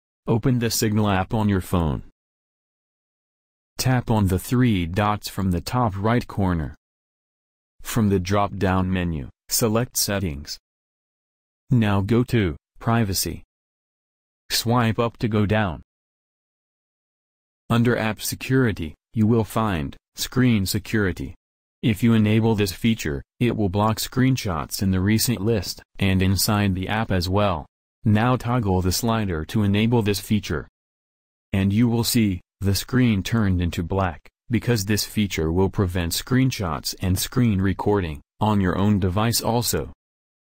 How to Enable Screen Security on Signal Messenger
In this short video I will show you, How to Enable Screen Security on Signal Messenger.